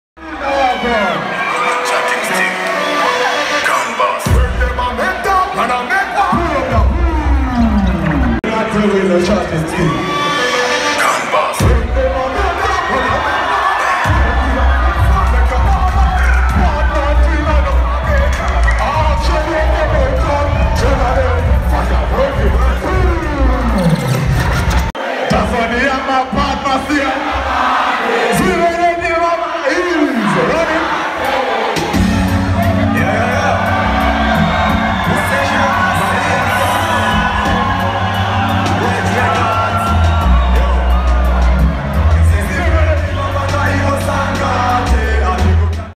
Malinga live @ Bunda University - Nov 2018
Malinga live on stage at Bunda College of Agriculture - Freshmans Show